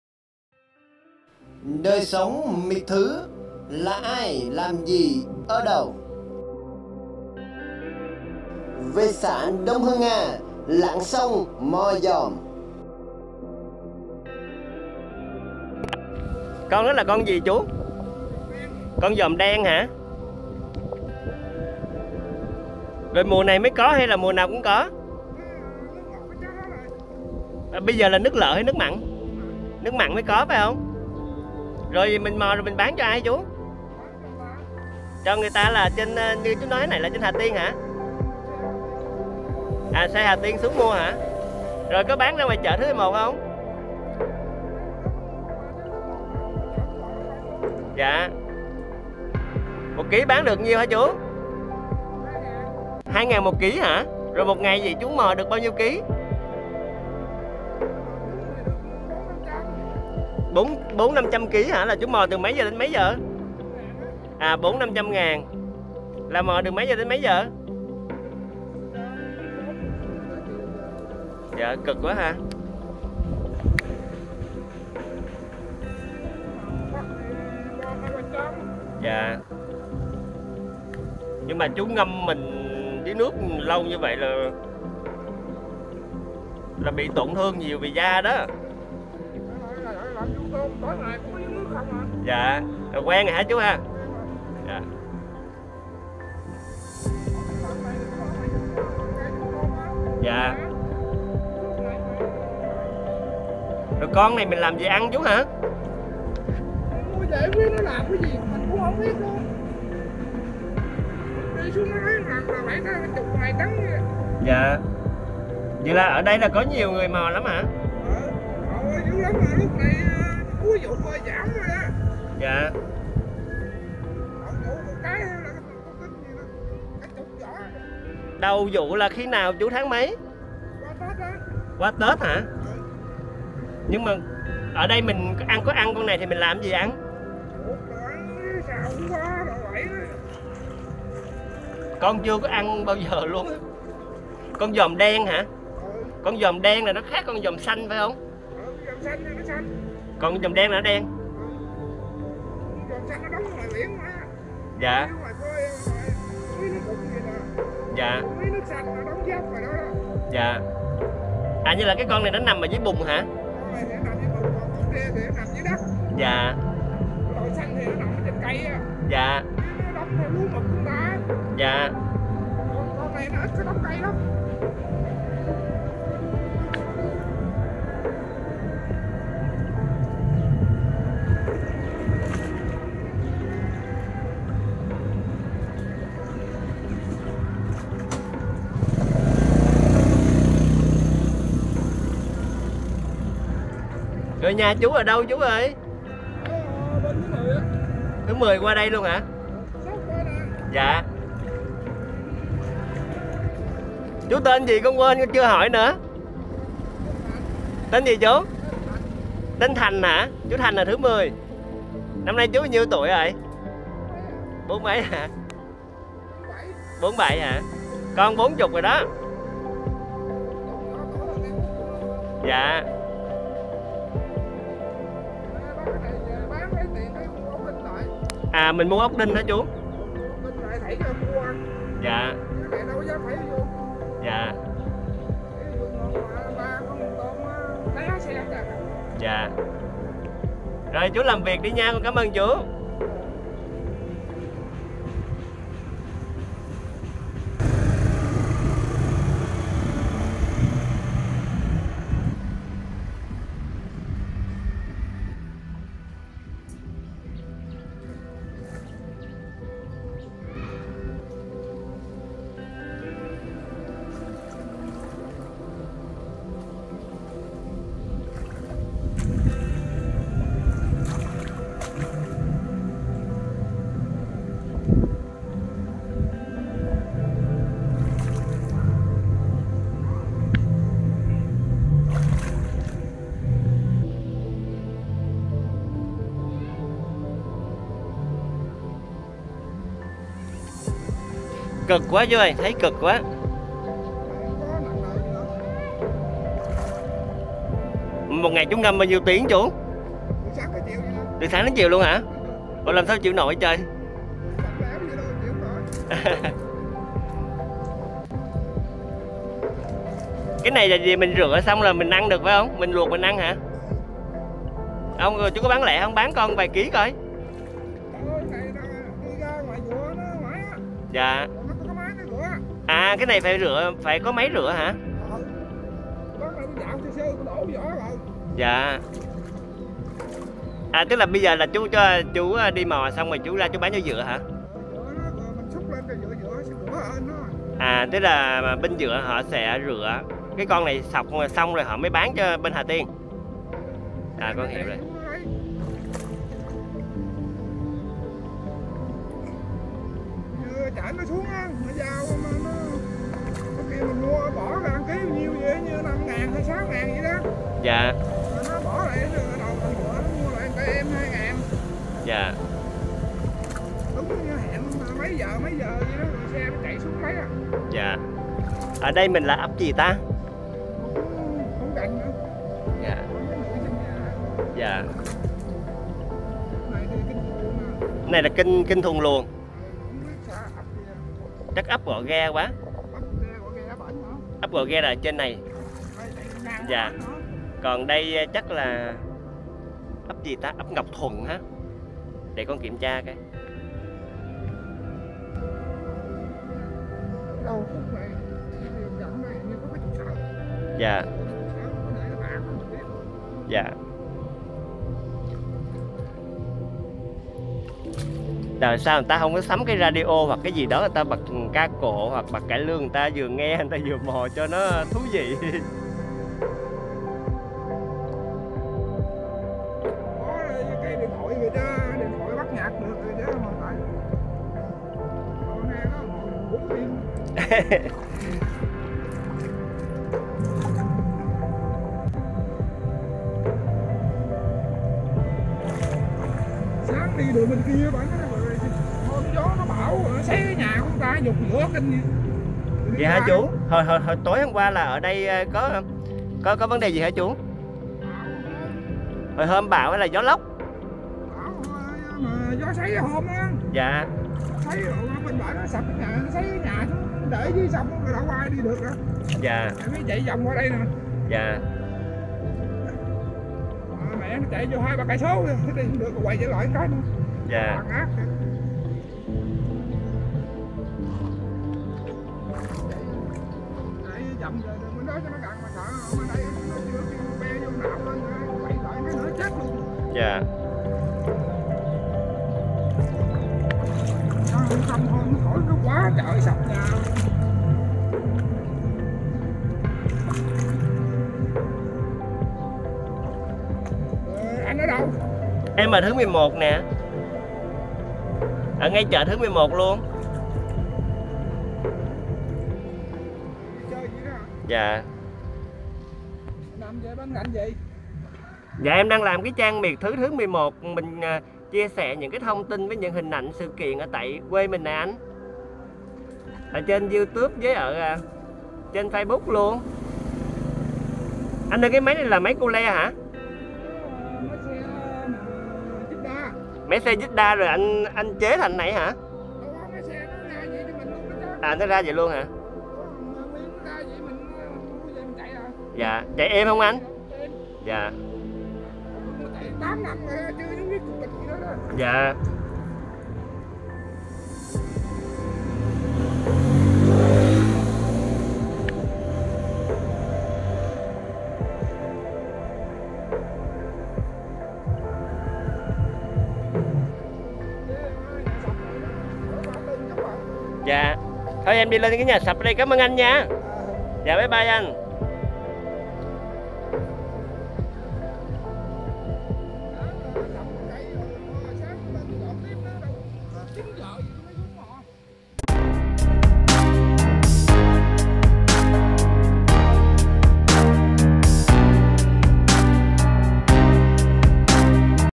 👉 LẶN SÔNG MÒ DÒM ĐEN vùng MIỆTTHỨ 📍
👉 ❛ĐỜI SỐNG❜ MIỆTTHỨ❗
        LÀ AI❓LÀM GÌ❓Ở ĐÂU❓
        Theo chân chú Thành ở Thứ 10 đi
                ➸ LẶN SÔNG MÒ DÒM (VẸM) ĐEN‼️
📍 Xã

❝ Miệt Thứ - Thứ11 hôm nay theo chân
chú Thành nhà ở Thứ10 chạy vỏ lãi sang
ấp Ngọc Thuận, xãĐôngHưngA ➸ lặn sông
mò dòm (vẹm) đen bán cho lái thu mua
từ HàTiên xuống Miệt Thứ ❞

               📽️ AkiQuang